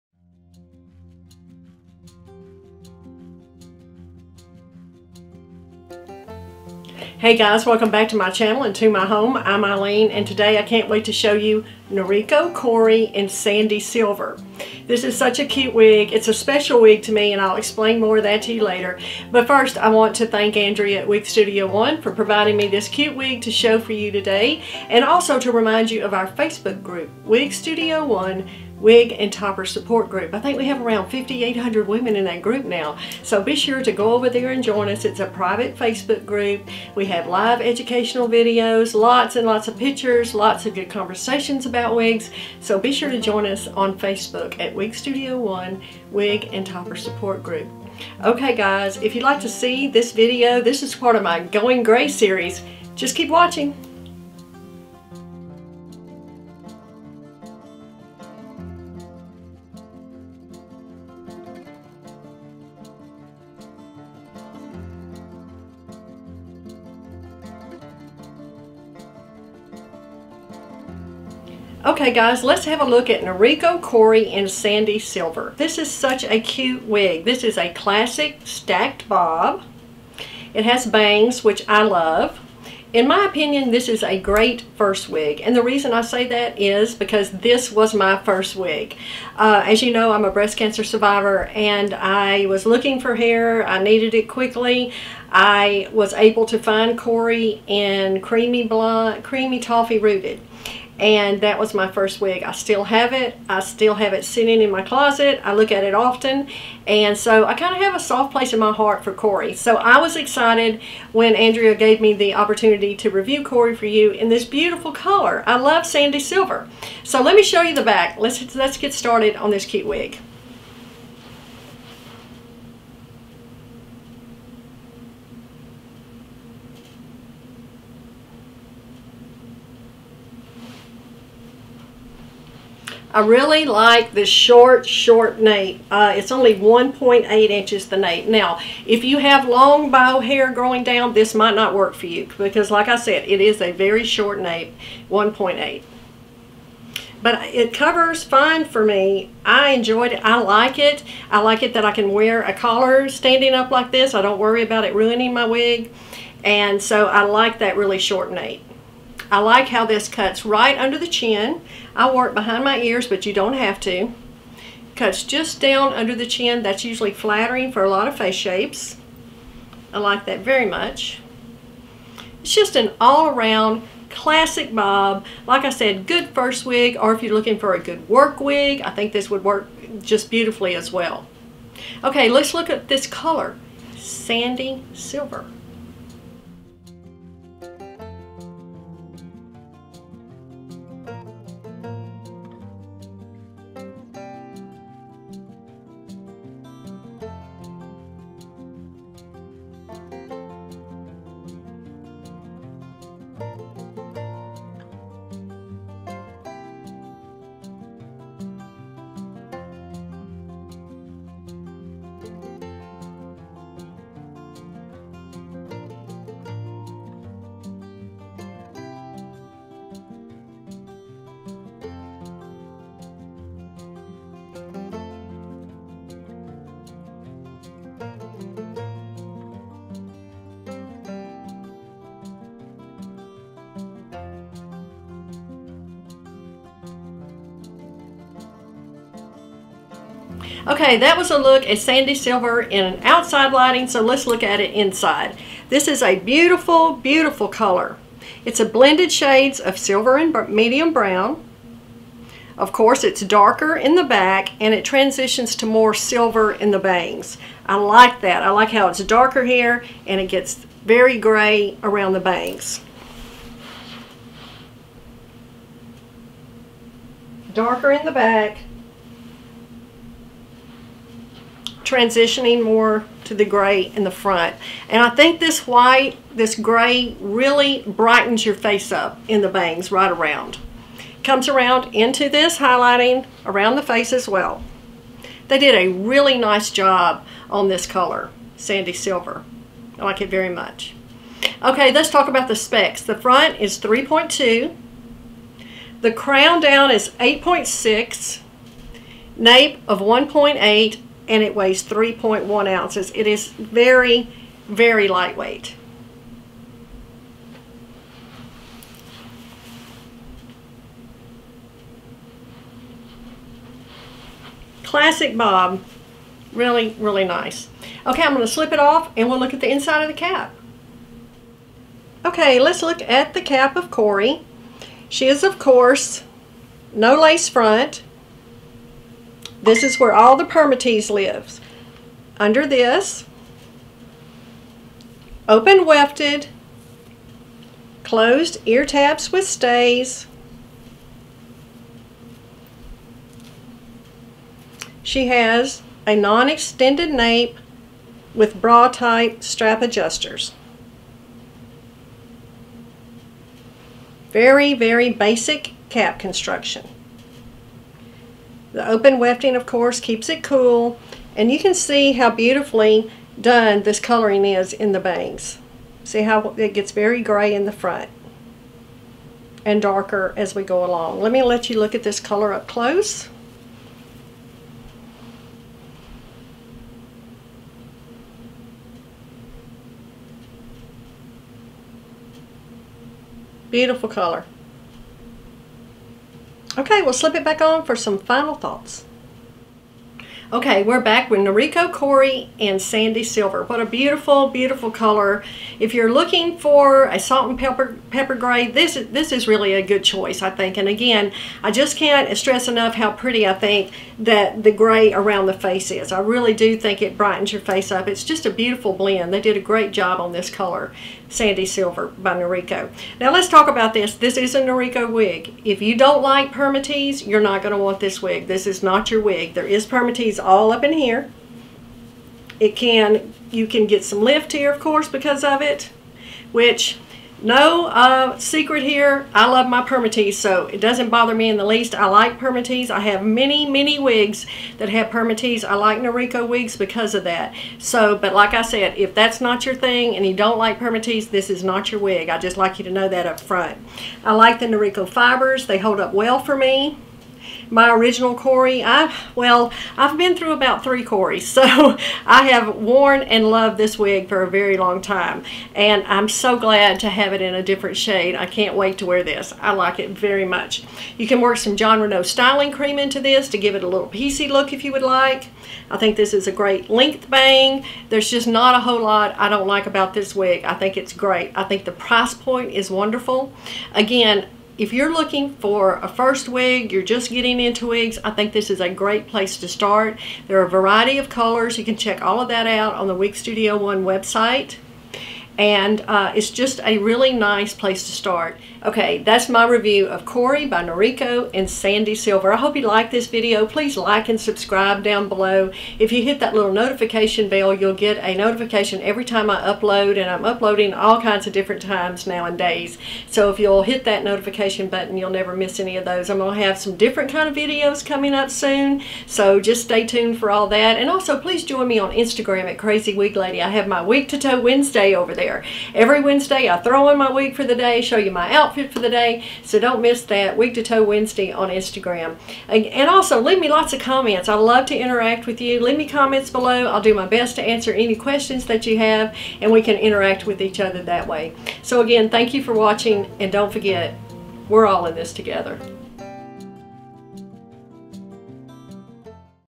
Noriko CORY Wig Review | Sandy Silver | Crazy Wig Lady
I love Noriko's Cory, it was my first wig and I have repurchased it again and again. I hope you enjoy this video!

The Cory Wig by Noriko is a classic bob hair style with a textured fringe. This short wig is also available in beautiful shades of grey.

SPECIAL FEATURES:
Open Cap Construction - The cap features open wefting that provides additional ventilation to help keep you cool.

Synthetic Hair Fiber - The synthetic fibers look and feel like human hair and are pre-styled and ready to wear right out of the box.

Collection:

Naturally Alternative Hair

Product Type:

Wig

Hair Type:

Synthetic Hair

Ideal Face Shape:

Round, Heart, Oval
Cap Construction:

Basic Cap

Cap Size:

Average

Product Weight:

3.1 oz

Approximate Length:

Front: 3.2" | Crown: 8.6" | Nape: 1.8"

My stats:
Head Circumference: 21 1/4"
Ear to Ear: 12 1/4"
Front to Back: 12 1/2"
Hairline to Bottom of Chin: 9"
Forehead: 2 1/4"

This video was written, produced and filmed in 2019 by Crazywiglady and is only authorized for use by Crazywiglady and wigstudio1.com. Any other copies or images taken from this video are unauthorized.

the blog consists of a written description of this wig, additional photos, color comparisons and my complete thoughts on the wig.

"Remember, we're all in this together"